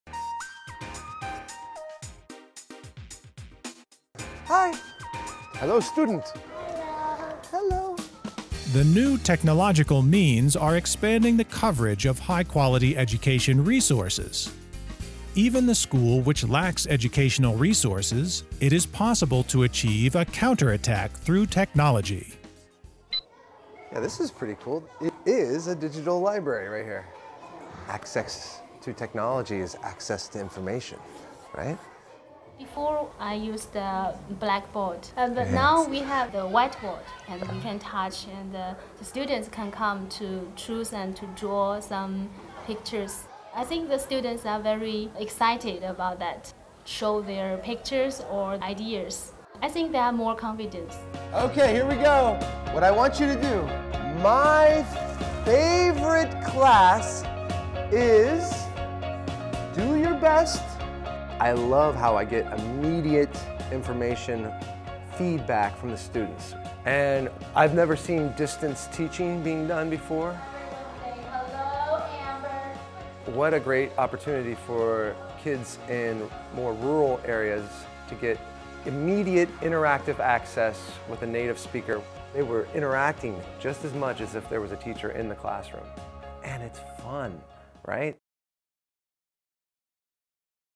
New technological means changing China's educational model
Technological education is one of the most favored subjects in Chinese elementary schools. Digital libraries and distance teaching are changing students' learning methods.